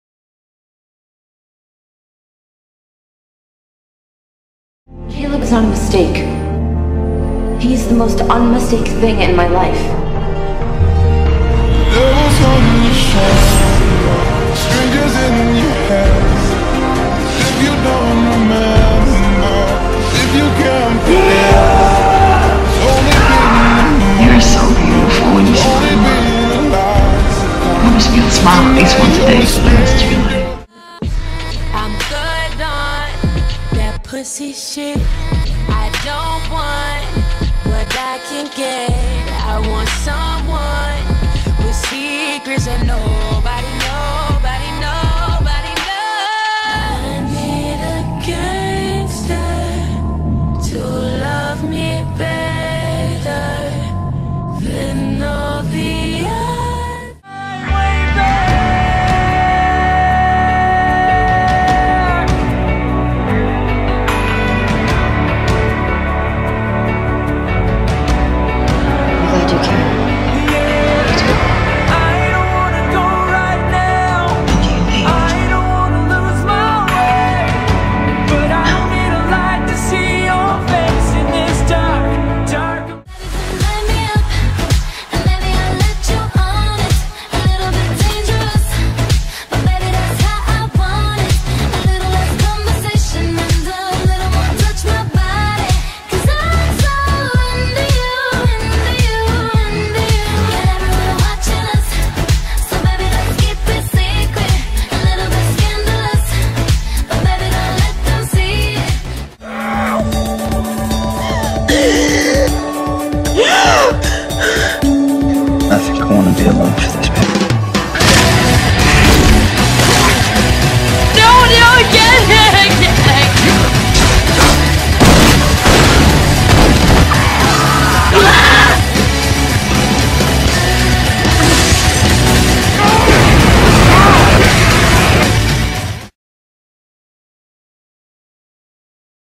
Promoting vidders #2
here are the promotions for those who asked that got a place in\
the CW contest that i co-judged along with Elo & Virginia!

-to the people that have asked for videos and or a banner and icon, it may be a while but I will get it done!!!